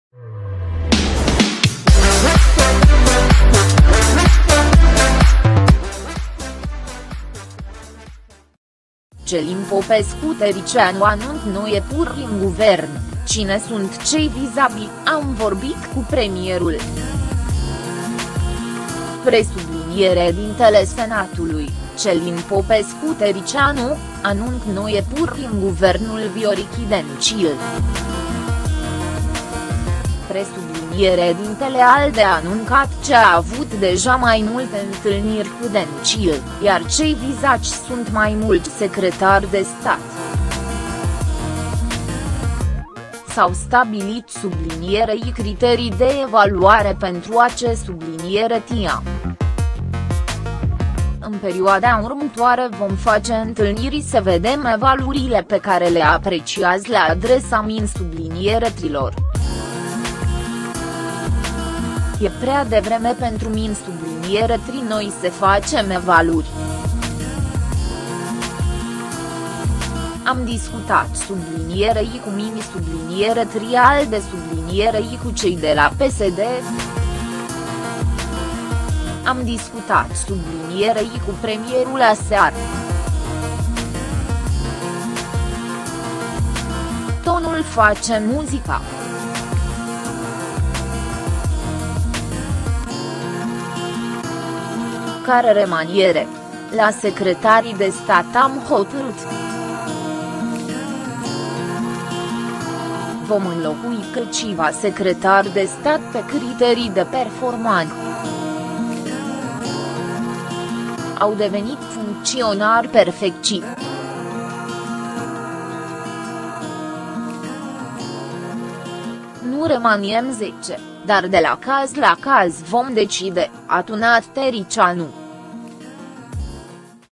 Călin Popescu Tăriceanu anunţă noi EPURĂRI în Guvern: Cine sunt cei VIZAŢI
Călin Popescu Tăriceanu anunţă noi EPURĂRI în Guvern: Cine sunt cei VIZAŢI - 'am vorbit cu premierul'